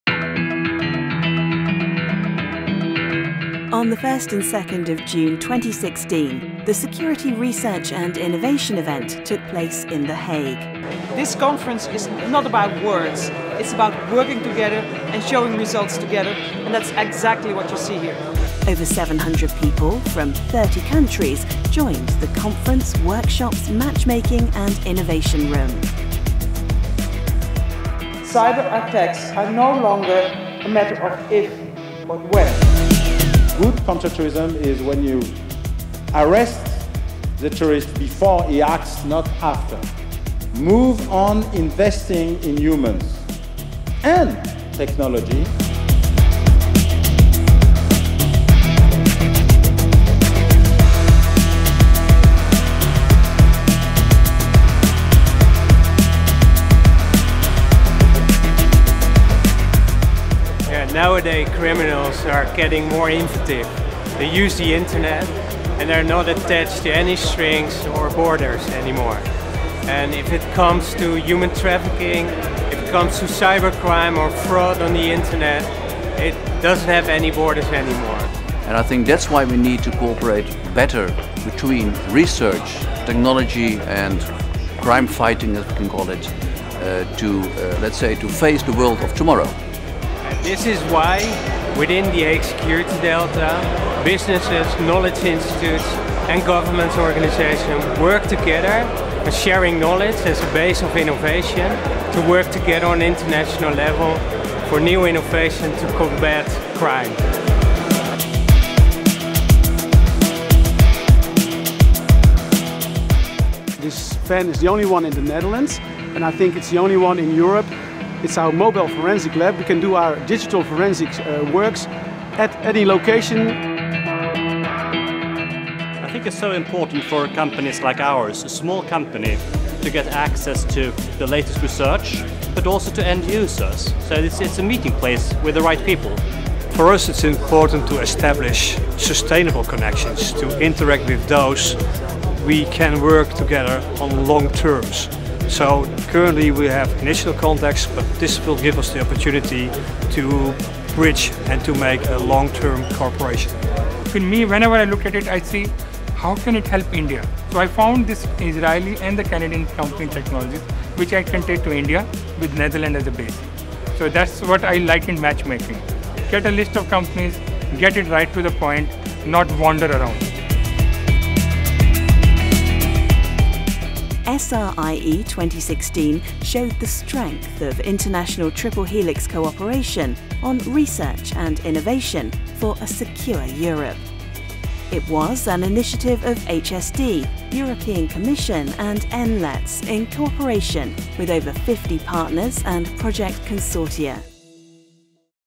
Security Research & Innovation Event 2016 – Recap of the event
Criminals and terrorists become more and more inventive, increasingly make use of the internet and are not bound to country borders. These developments create international challenges for counterterrorism, border control and the prevention of cybercrime and fraud. International cooperation, knowledge sharing, research and the creation of innovative detection and security technologies are key to solve these challenges. That is why within the Dutch security cluster ‘The Hague Security Delta’, businesses, governments and knowledge institutions work together, share knowledge and set up knowledge bridges with international security clusters to create innovative security solutions. To facilitate this international and triple helix cooperation, HSD, together with the European Commission and the European Network of Law Enforcement Technology Services, organised the Security Research and Innovation Event on 1 and 2 June 2016 in The Hague.

The event consisted of a conference, workshops, matchmaking programme and Innovation Room and was opened by Arie IJzerman (Deputy Director General Law Enforcement and Administration) on behalf of the Minister of Security and Justice and by Deputy Mayor of the City of The Hague Van Engelshoven. It was the place where over 700 international policy makers, entrepreneurs, corporates, academics and practitioners came together to discuss the current European security challenges and get access to knowledge, innovations and market. The event provided the opportunity to find international partners to create innovative innovations for a secure society that are eligible for funding from the European Commissions Horizon 2020 Programme. On top of that, practitioners like the police gave insights in their concrete security challenges, needs and dreams. These openings for innovative solutions were gained by the ENLETS networks.

Besides the EC Security Research Conference, another highlight of the SRIE2016 was the Innovation Room, where 35 innovative security solutions were demonstrated. It was opened by Ingrid van Engelshoven, deputy mayor of the city of The Hague. During the successful matchmaking programme 137 participants from 22 countries participated in 240 bilaterial meetings resulting in concrete cooperations.

SRIE2016 showed the strength of international triple helix cooperation on research and innovation for a secure Europe. It was an initiative of HSD, EC  and ENLETS in cooperation with 50 partners.